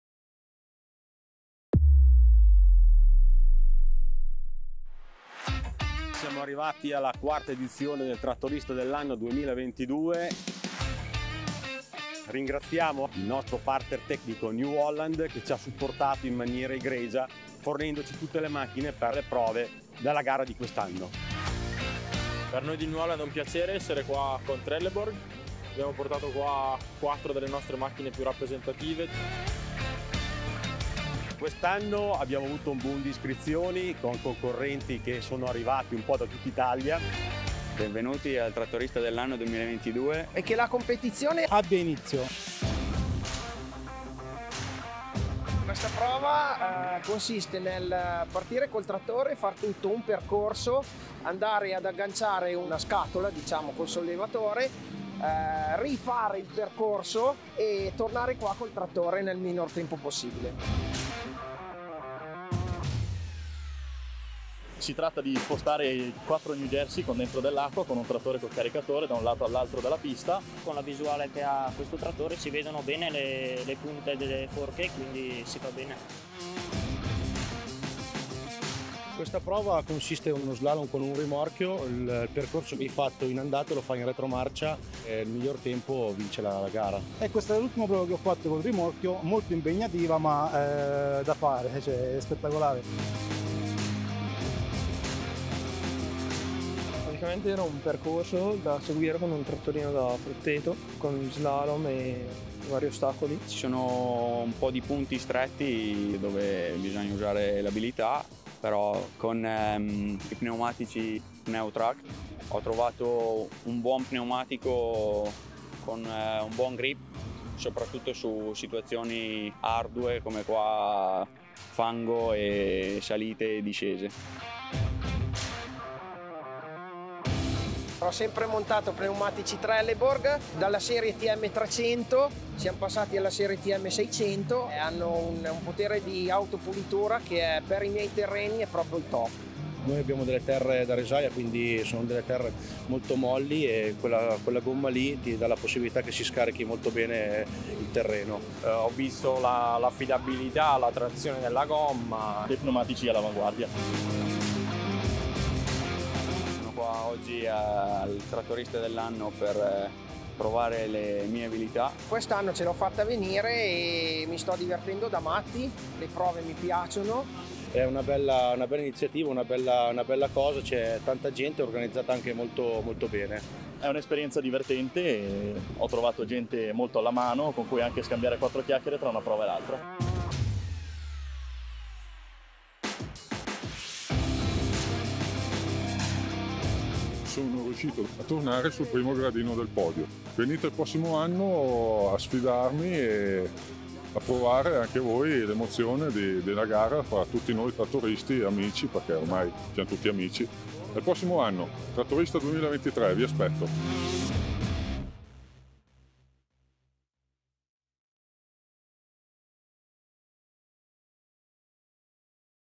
Trelleborg - Trattorista dell'anno 2022
Il vincitore della quarta edizione della competizione è Alberto Allegretti, da Pegognaga, in provincia di Mantova.

Il Trattorista dell'anno è la competizione tra conduttori di trattori organizzata da Trelleborg, giunta ormai alla quarta edizione.

L'evento si è svolto il 19 Novembre al Cremona Circuit (Via Strada Giuseppina, 2, 26040 San Martino del Lago CR), dove i trattoristi si sono messi in gioco per godersi una giornata all'insegna di una sana competizione e per aggiudicarsi il titolo di Trattorista dell'anno 2022.